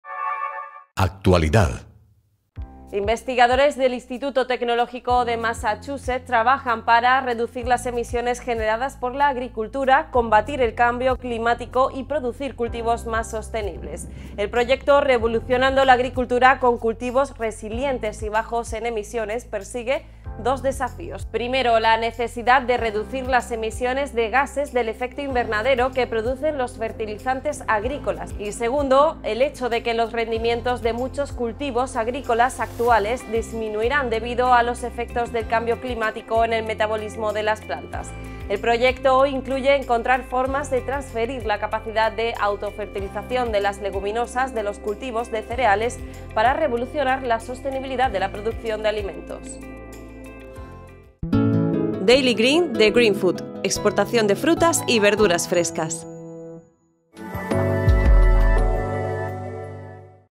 Autofertilizantes para combatir el cambio climático.
Investigadores del Instituto Tecnológico de Massachusetts trabajan para reducir las emisiones generadas por la agricultura, combatir el cambio climático y producir cultivos más sostenibles.
El proyecto, 'Revolucionando la agricultura con cultivos resilientes', persigue dos desafíos: primero, la necesidad de reducir las emisiones de gases de efecto invernadero que producen los fertilizantes agrícolas y, segundo, el hecho de que los rendimientos de muchos cultivos agrícolas actuales disminuirán debido a los efectos del cambio climático en el metabolismo de las plantas.
El proyecto incluye encontrar formas de transferir la capacidad de autofertilización de las leguminosas a los cultivos de cereales para revolucionar la sostenibilidad de la producción de alimentos.